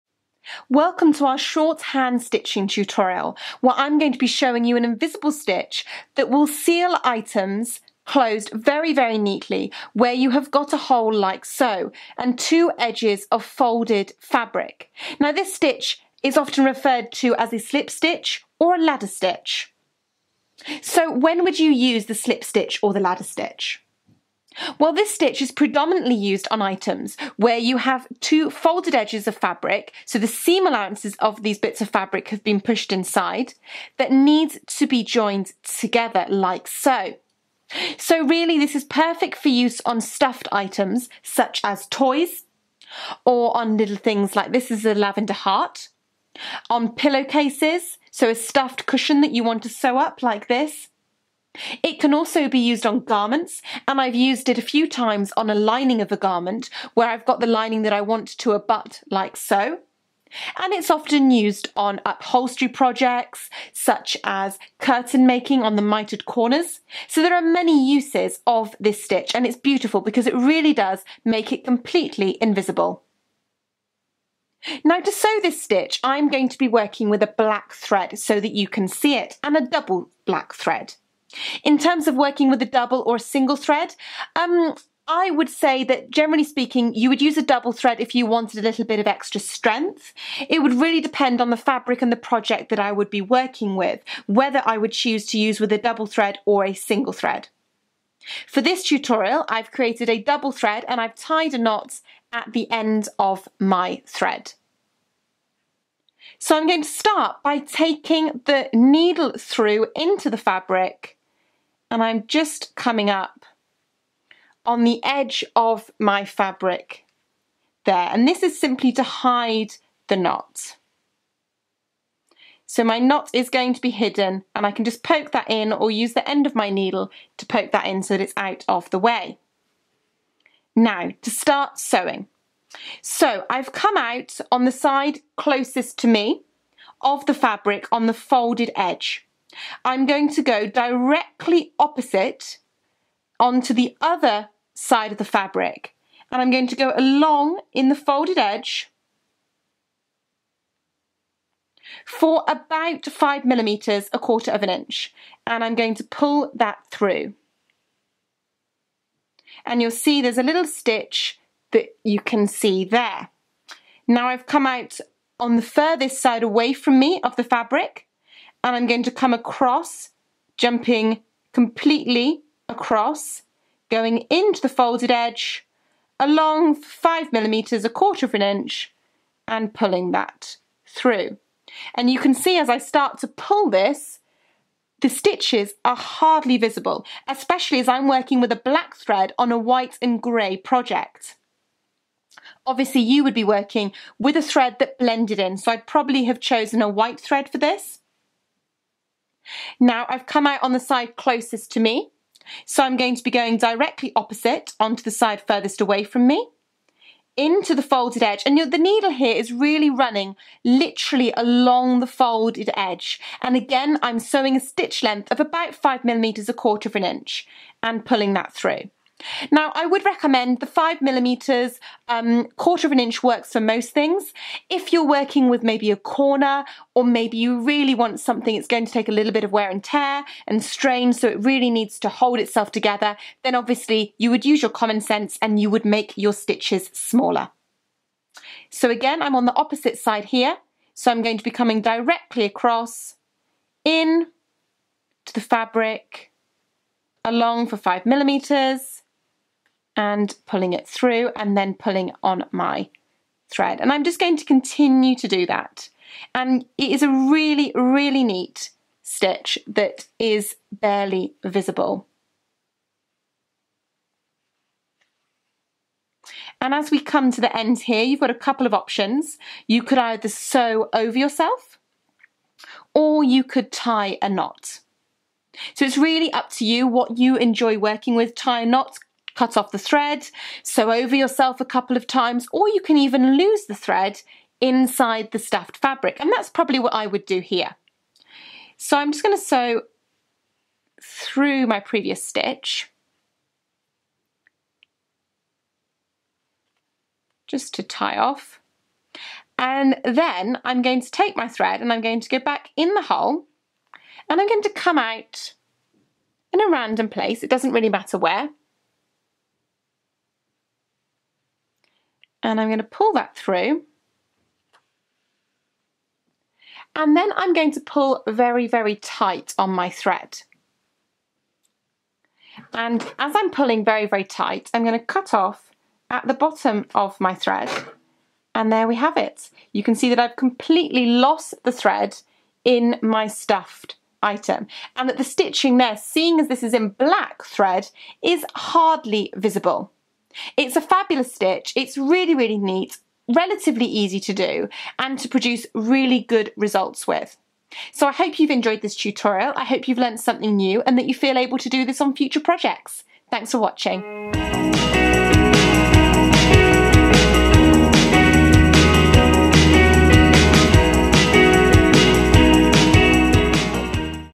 How To: Invisible Stitching (Slip Stitch / Ladder Stitch)
Learn how to sew an invisible hand stitch, often referred to as the slip stitch or ladder stitch.

A wonderful and useful skill for a variety of sewing situations. This hand stitch is perfect for use when sewing up holes in items such as stuffed toys, decorations, pillows or cushion covers. It is also used to sew the lining in certain garments and the mitred corner on curtains.